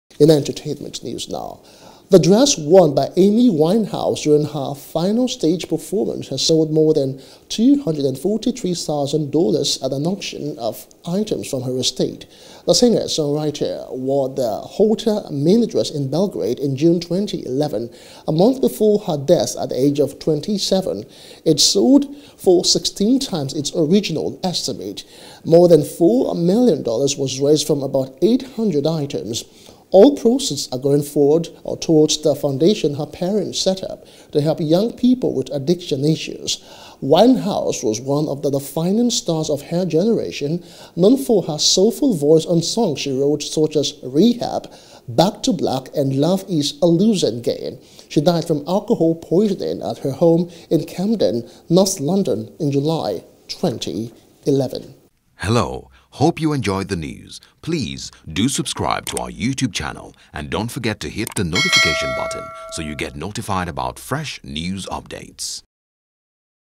Amy Winehouse's Items Raise Millions At Auction | ENTERTAINMENT
The dress worn by Amy Winehouse during her final stage performance has sold for more than $243,000 at an auction of items from her estate.